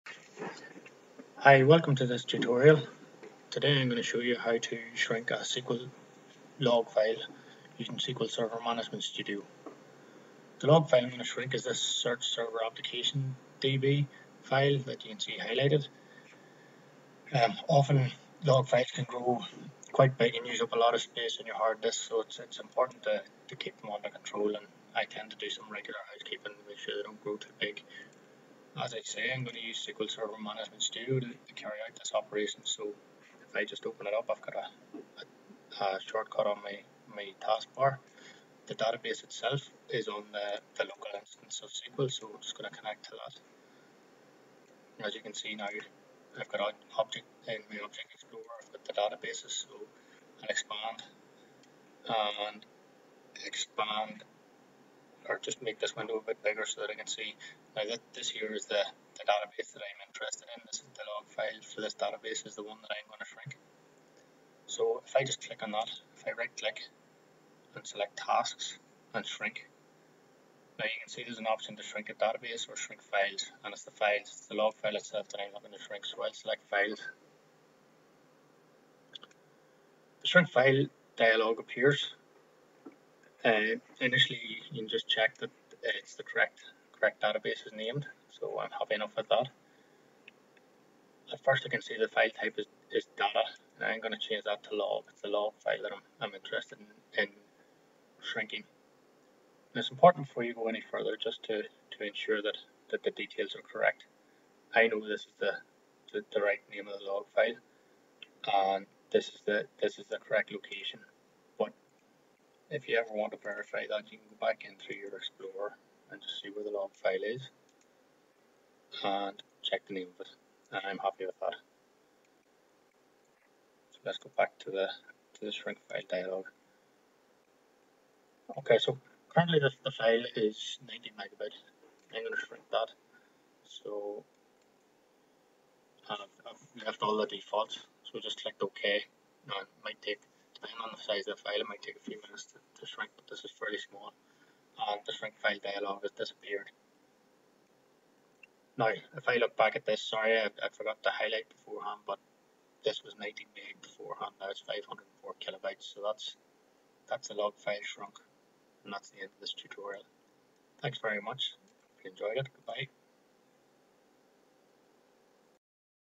Shrink A SQL Server Log File
Tutorial demonstrating how to shrink a SQL Server database log file using SQL Server Management Studio(SSMS). The version of SQL used for the tutorial is SQL Server 2008 R2.